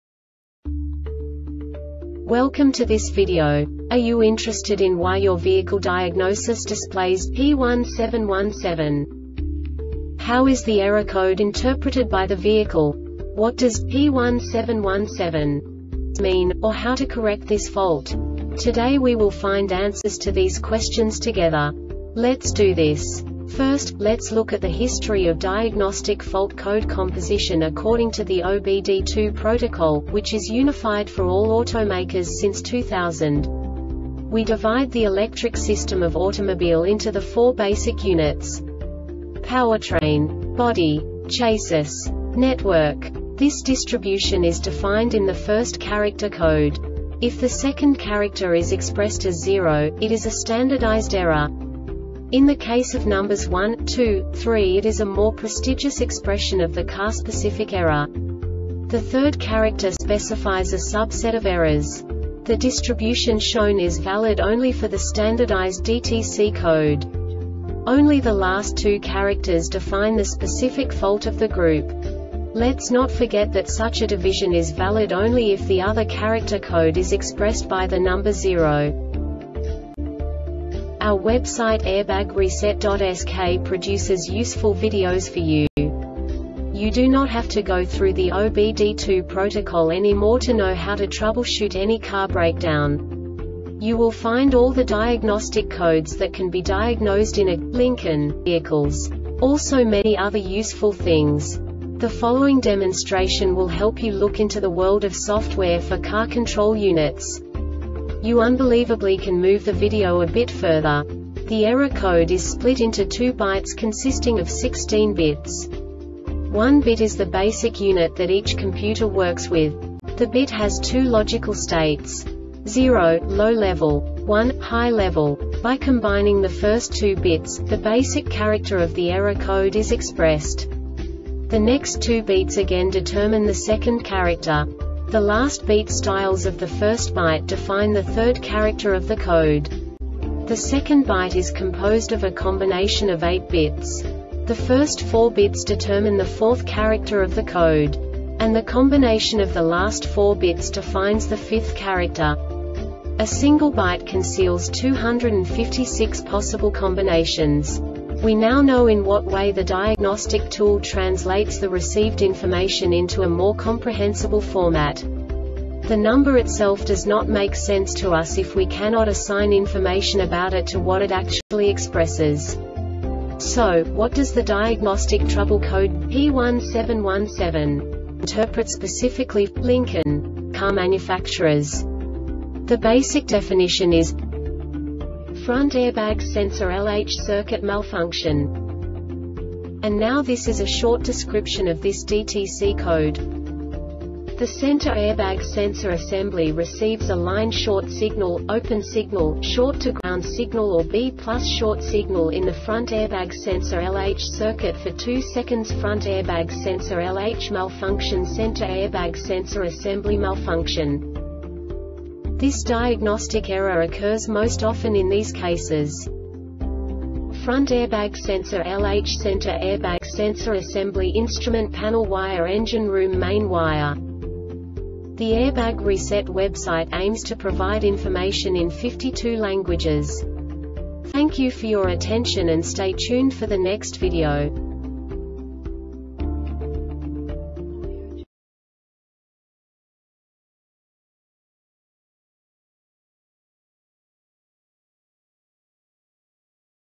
DTC Lincoln P1717 Short Explanation
Contents:
0:21 Basic DTC analysis according to OBD2 protocol standard.
1:48 Insight into programming
2:58 A diagnostically understandable description of the Diagnostic Trouble Code
3:05 Basic error definition

Make:
Lincoln
Code:
P1717
Definition:
Front Airbag Sensor LH Circuit Malfunction
Description:
The center airbag sensor assembly receives a line short signal,open signal,short to ground signal or B+ short signal in the front airbag sensor LH circuit for 2 seconds.Front airbag sensor LH malfunction Center airbag sensor assembly malfunction
Cause:
 Front airbag sensor LH Center airbag sensor assembly Instrument panel wire Engine room main wire